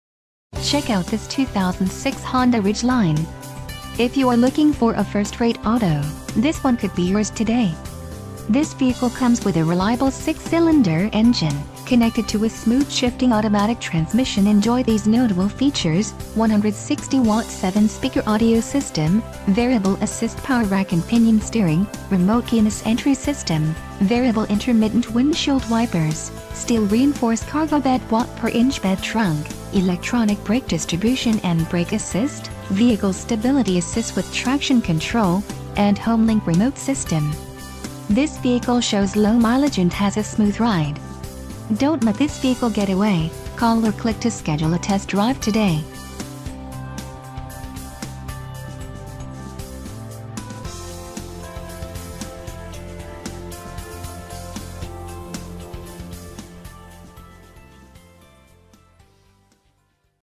2006 HONDA RIDGELINE - Pahrump Valley Auto Plaza - Pahrump,
Check out this 2006 HONDA RIDGELINE. If you are looking for a first rate auto, this one could be yours today. This vehicle comes with a reliable 6 cylinder engine, connected to a smooth shifting automatic transmission.. Enjoy these notable features: 160-Watt 7-Speaker Audio System, Variable-Assist Power Rack and Pinion Steering, Remote Keyless Entry System, Variable Intermittent Windshield Wipers, Steel-Reinforced Cargo Bed w/In-Bed Trunk, Electronic Brake Distribution and Brake Assist, Vehicle Stability Assist w/Traction Control, and HomeLink Remote System. This vehicle shows low mileage and has a smooth ride. .